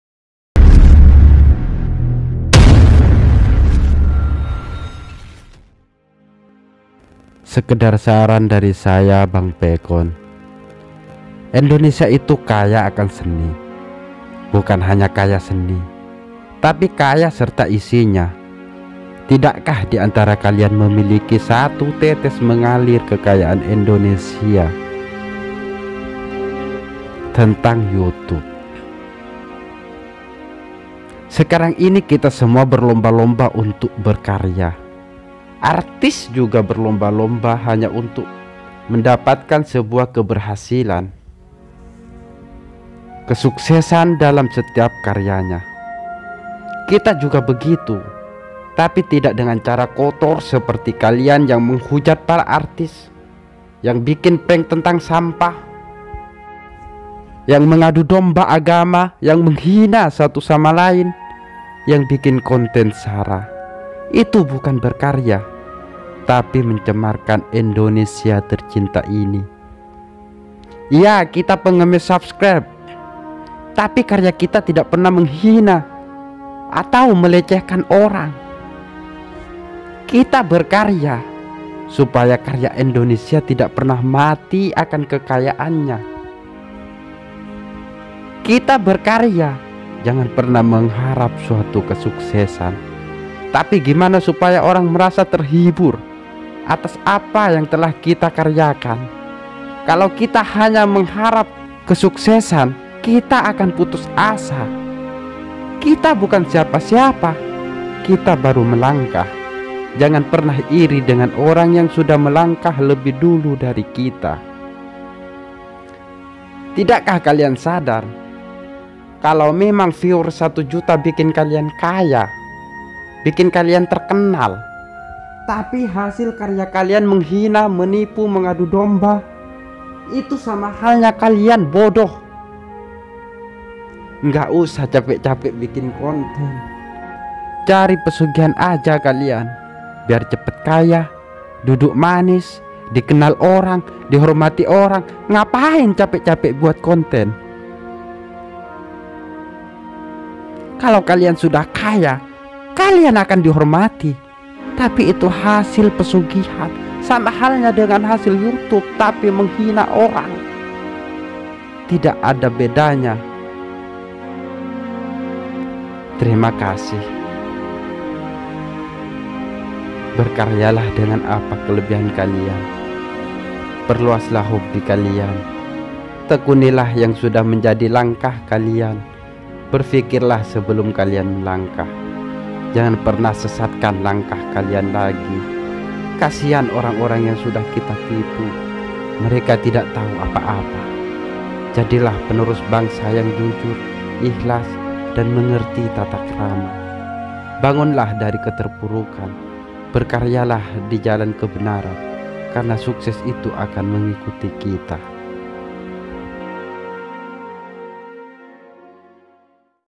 Pesanku buat para youtuber indonesia
Jadilah penerus bangsa yang jujur ikhlas
Dan mengerti tatak rama
Bangun lah dari keterpurukan
Berkaryalah di jalan kebenaran.
Karna sukses itu akan mengikuti kita.
Penulis : bang pekon

Buat yang uda mampir thanks
Oy jangan sampai lupa tekan tombol 👇

Selamat menonton
Karya : bang pekon
Editor : bang pekon

youtuber prank sampah di gebukin,